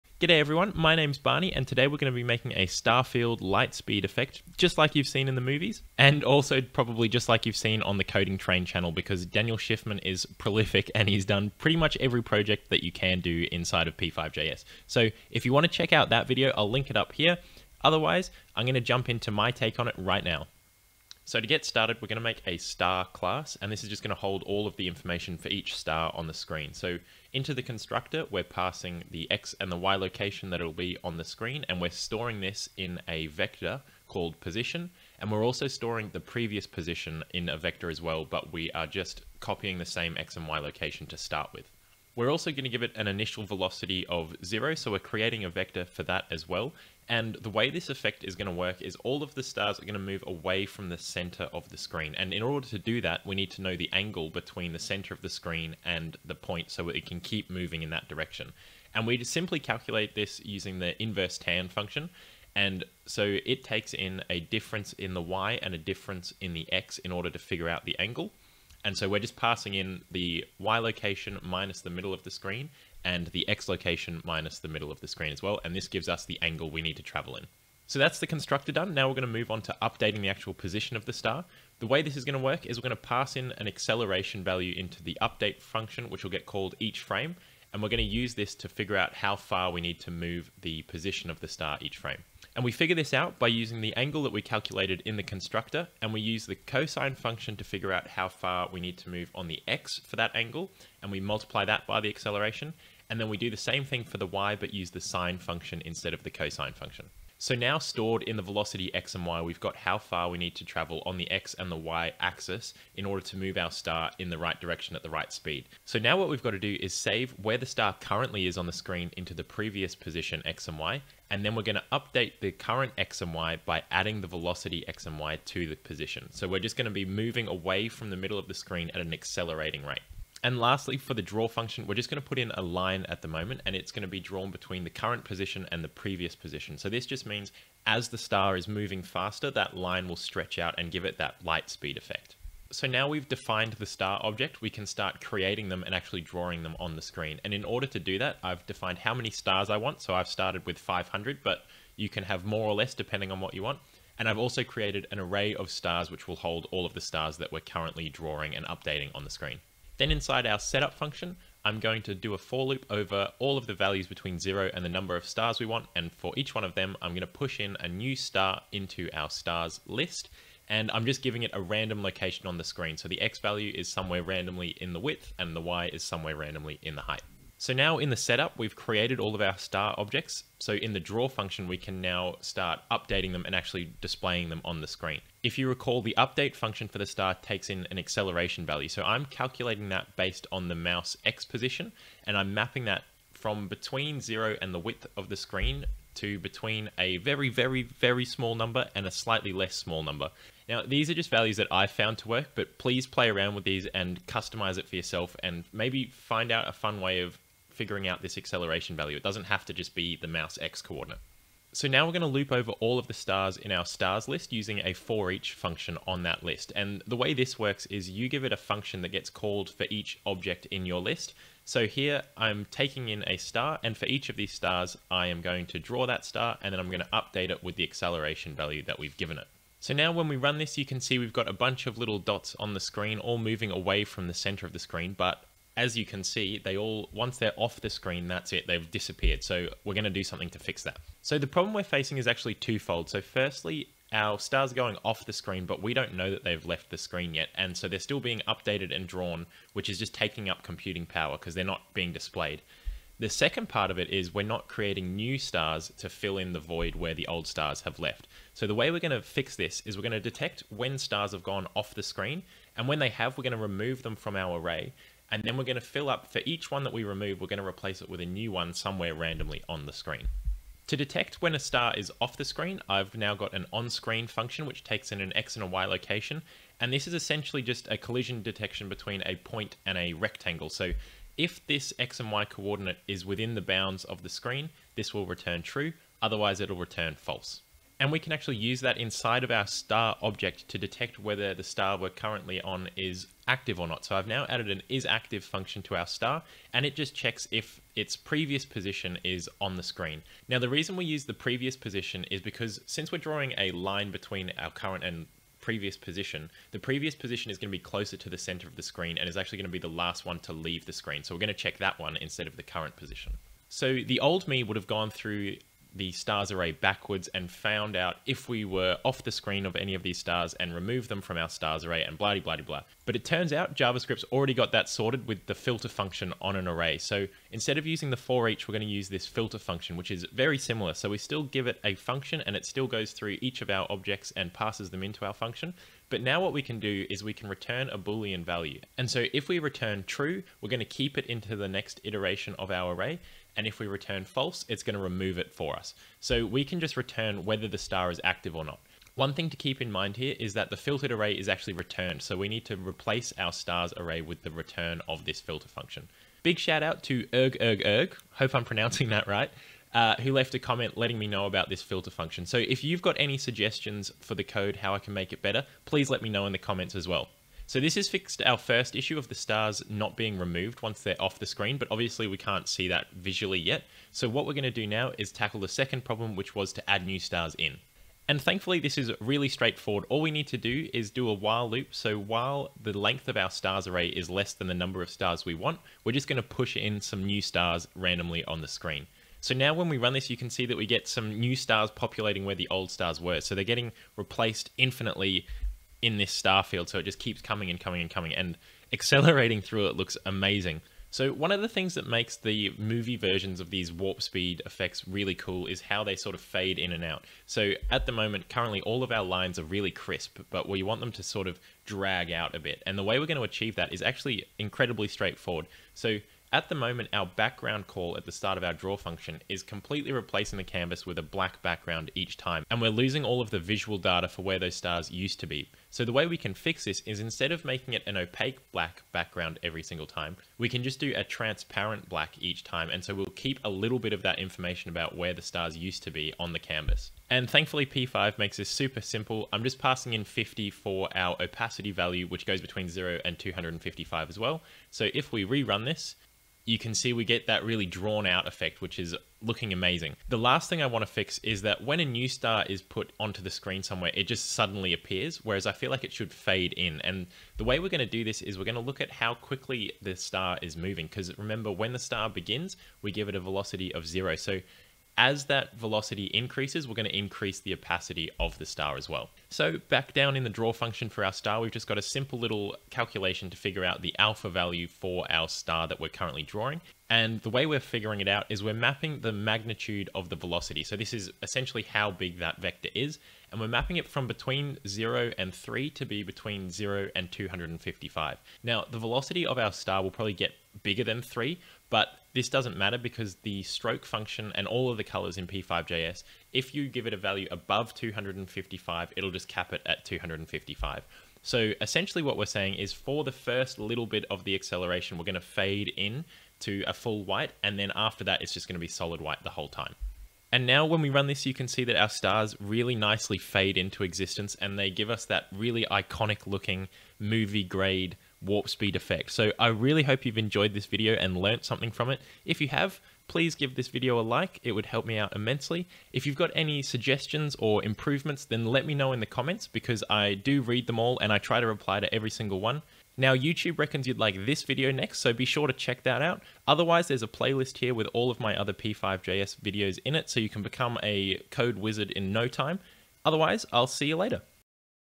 Star Field Hyperdrive Light Speed Effect
The star field light speed effect is an iconic effect often seen in movies (eg Star Wars) and gives an amazing sense of depth considering we are simply drawing lines on the screen!

0:00 Intro
0:20 Star class
2:21 Adding random stars
4:00 Removing stars that have left the screen
5:57 UHRG UHRG UHRG
6:07 Adding new stars to replace the old ones
6:40 Giving the stars a trail
7:30 Fading in the new stars
8:30 Final product